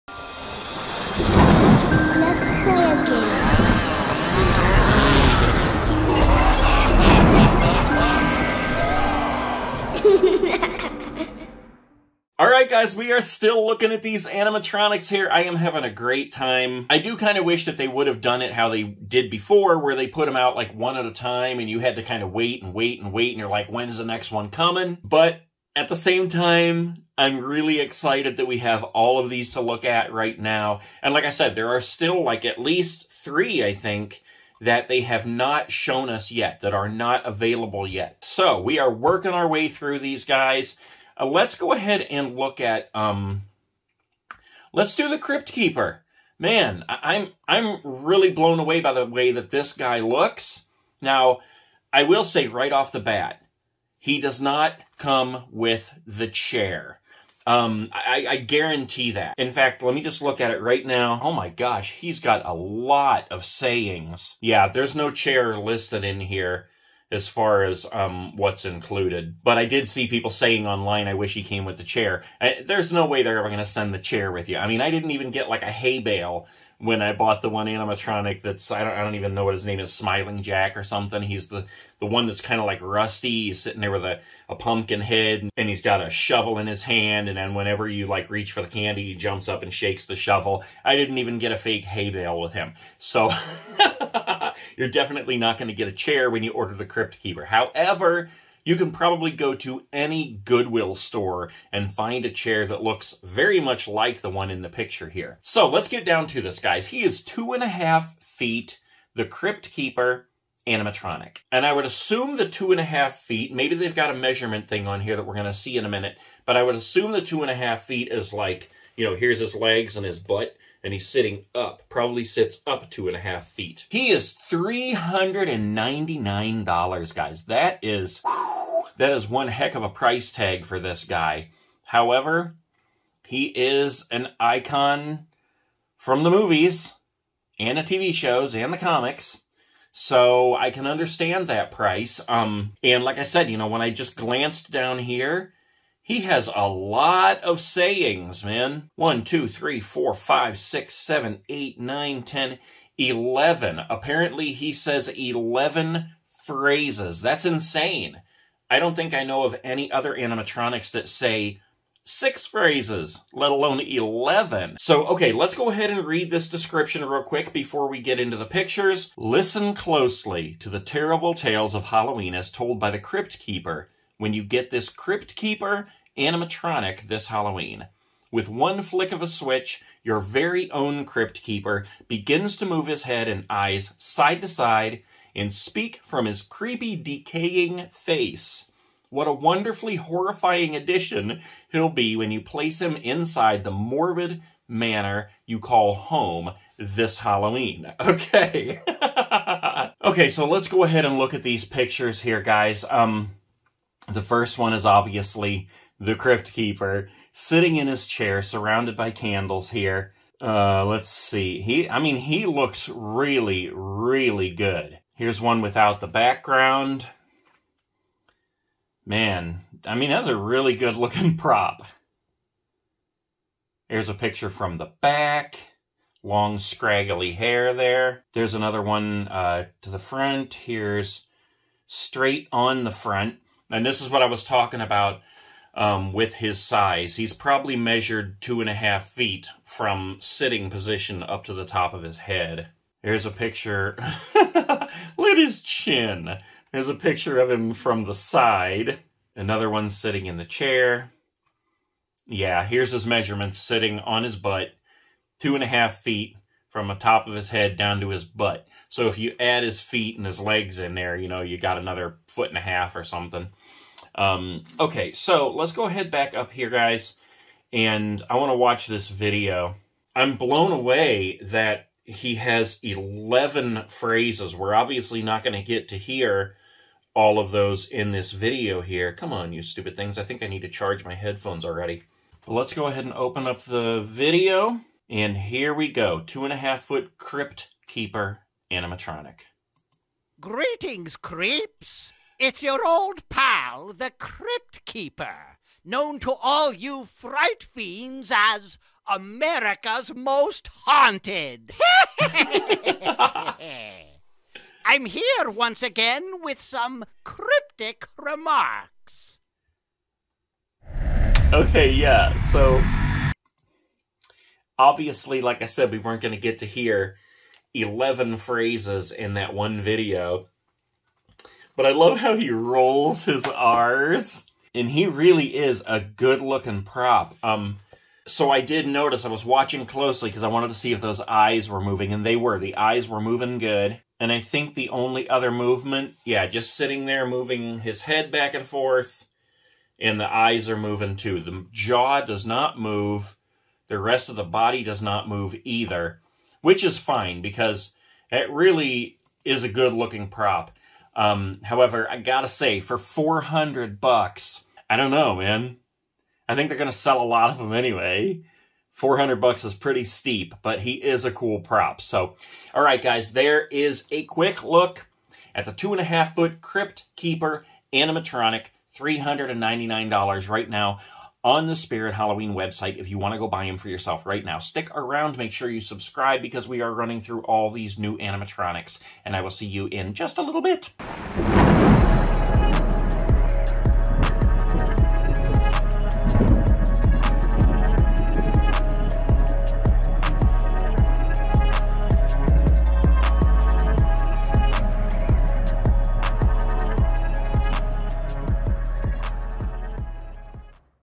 SPIRIT HALLOWEEN 2023 CRYPT KEEPER ANIMATRONIC TALES FROM THE CRYPT PROP REACTION SNEAK PEEK
TALES FROM THE CRYPT! Today we're looking at the Crypt Keeper animatronic from Spirit Halloween new for the 2023 Halloween season!

SEND FAN MAIL TO NATE!
        That Nate Guy On YouTube